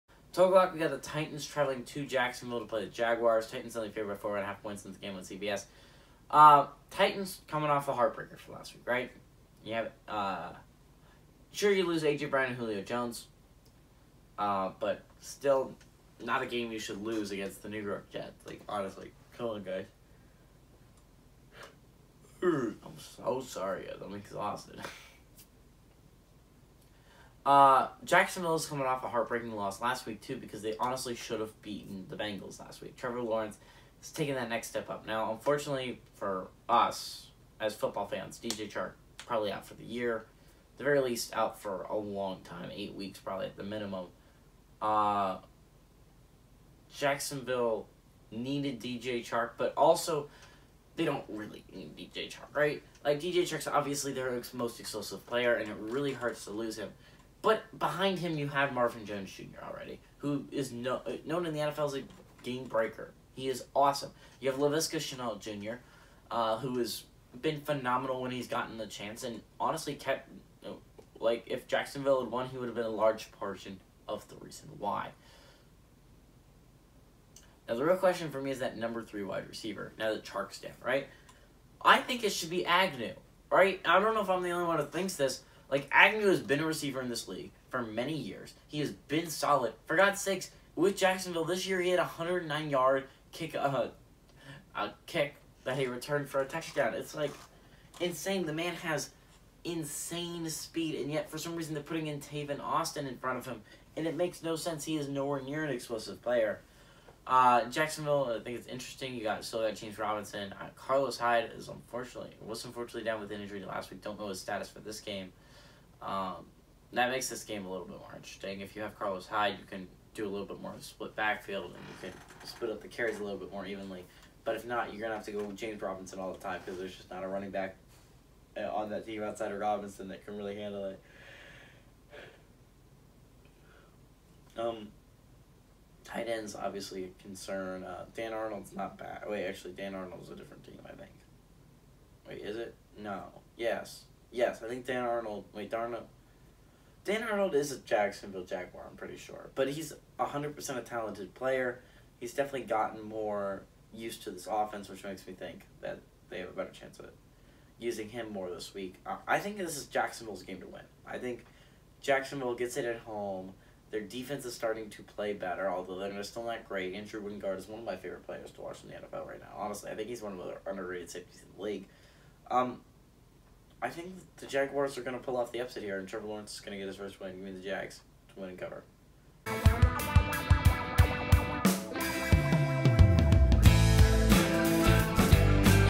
Titans @ Jaguars
In this video, GGB predicts one of the biggest games of the week. Will the Titans get their second AFC South win or will the Jags pick up their first win? The only way to know is to watch the video.